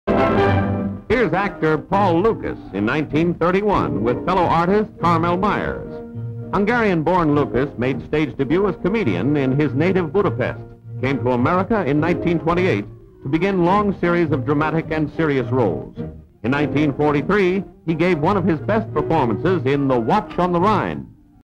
1931 Actors Paul Lukas and Carmel Myers
Reel #: 10023 TC In: 012436

1931 Fellow actors Paul Lukas and Carmel Myers, dressed up and palling around with a dog.